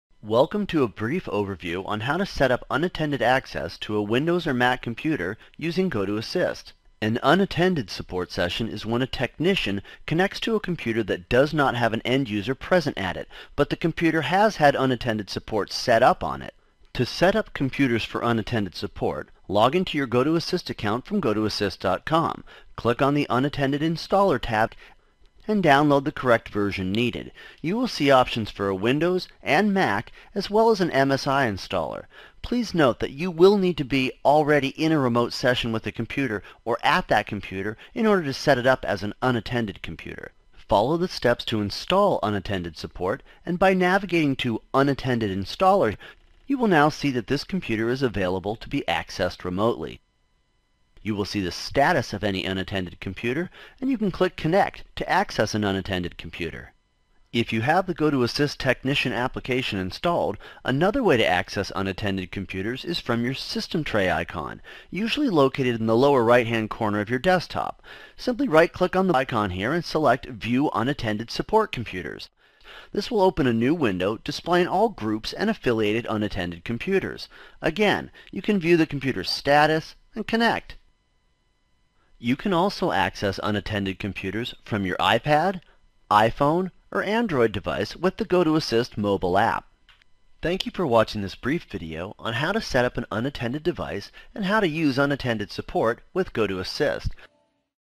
GoToAssist: Remote Support -- Unattended Support
Setting up an unattended computer with GoToAssist.